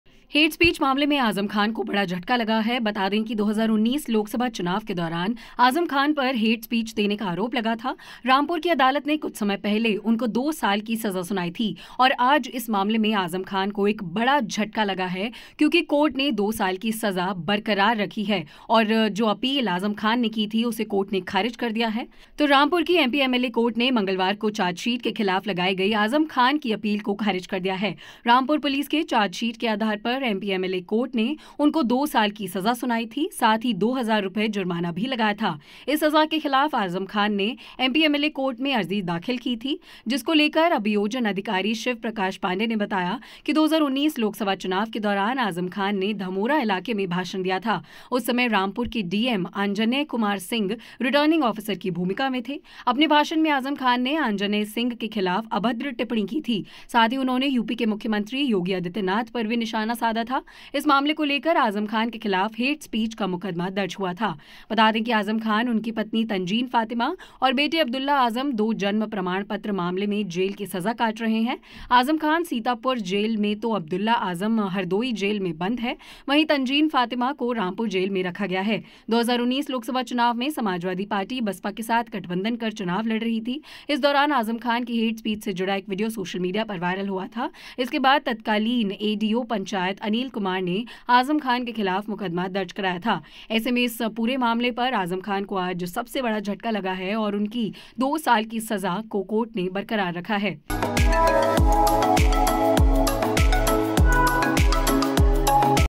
Azam Khan News:कोर्ट से मिला आजम खान को फिर से बड़ा झटका, 2 साल की सुनाई सजा
Azam Khan: कोर्ट से मिला आजम खान को फिर से बड़ा झटका, 2 साल की सुनाई सजा